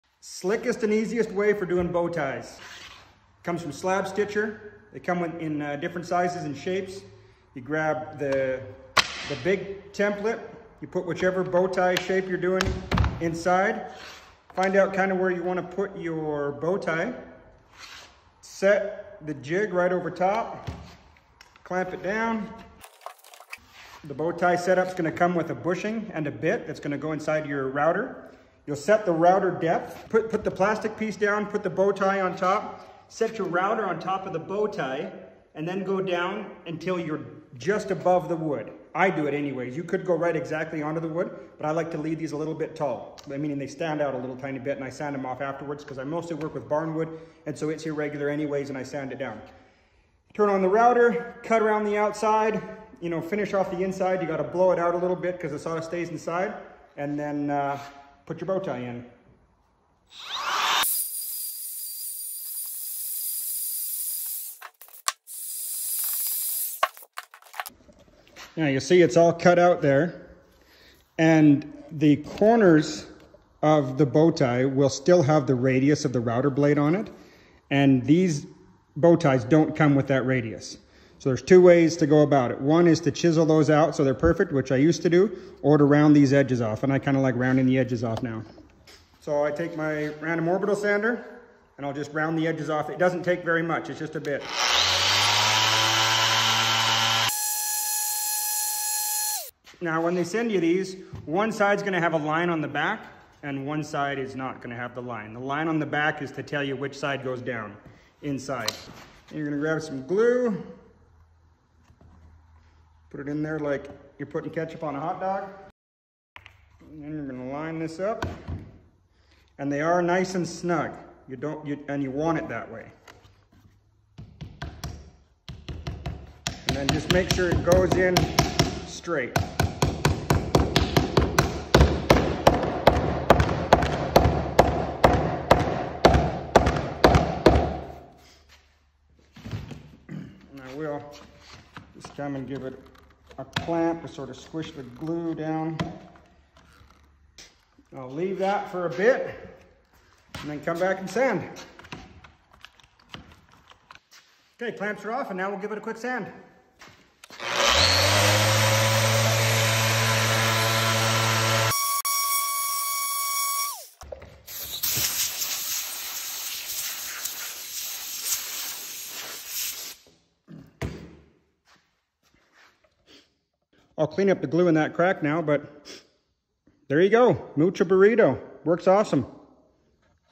How to do bow ties with Slabstitcher
Quickest and cleanest way to put bow ties in your woodwork projects. Dusty shows how to use slabstitcher to put bow ties in wood work projects

To visit Dustys Amazon page for tools and products he uses and recommends